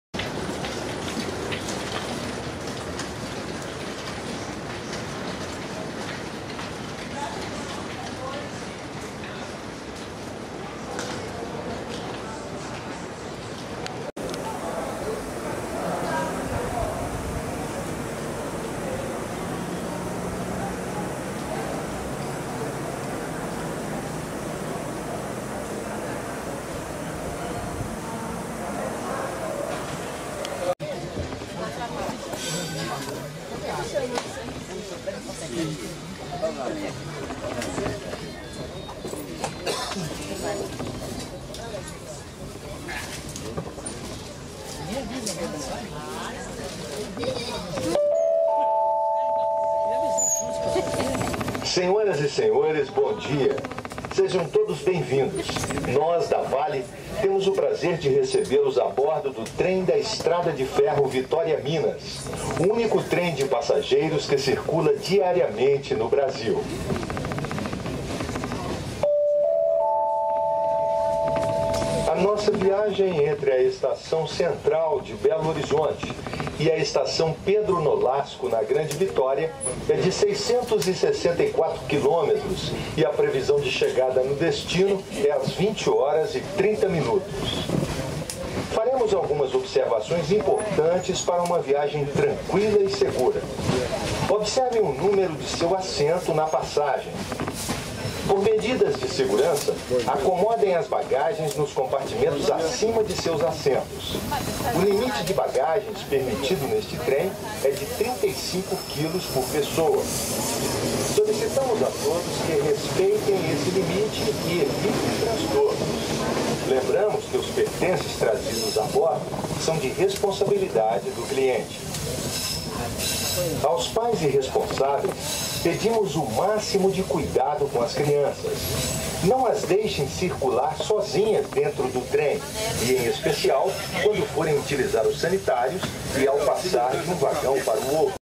Trem de passageiros Belo Horizonte - Vitória - Em busca dos caminhos do Brasil
Projeto "Em busca dos caminhos do Brasil".
Saída do Trem da Estação Central de Belo Horizonte (MG) com destino a Vitória (ES). Nosso percurso vai até Governador Valadares (MG), cidade à margem esquerda do Rio Doce, na metade do trajeto, que também é servida pela rodovia BR-116 conhecida como Rio-Bahia. No video, travessa pela Grande Belo Horizonte (capital e cidades ao seu entorno). A Rio-Bahia foi a primeira rodovia de ligação entre o Nordeste e o Sudeste e Sul do Brasil.